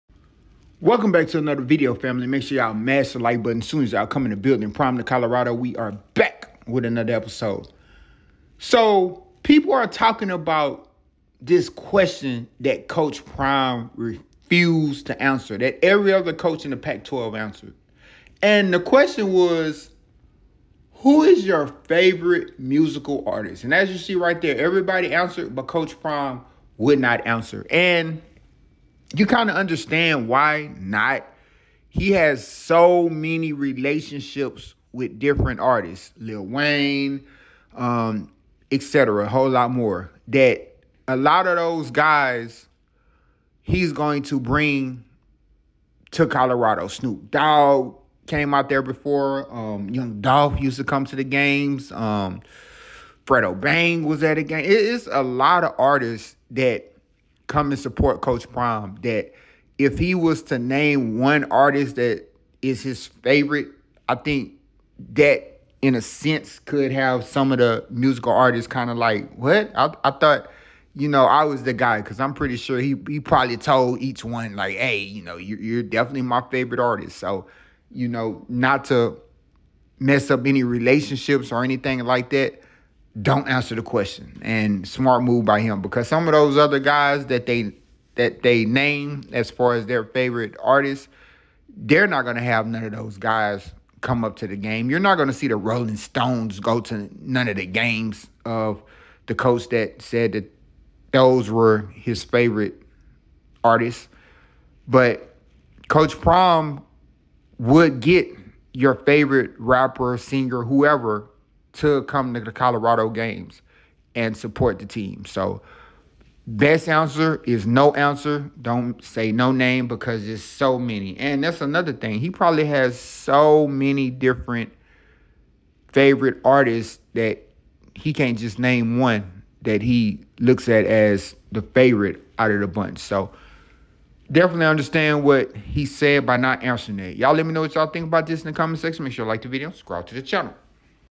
Coach Prime Refused To Answer This Question Every Other Pac 12 Coach Answered ‼️
The Colorado Buffaloes made a great hire by bringing in coach prime, make sure to continue to tune into this channel for the latest info

Coach Prime Has Been Doing A Fantastic job helping the Colorado Buffaloes Turn Around There Program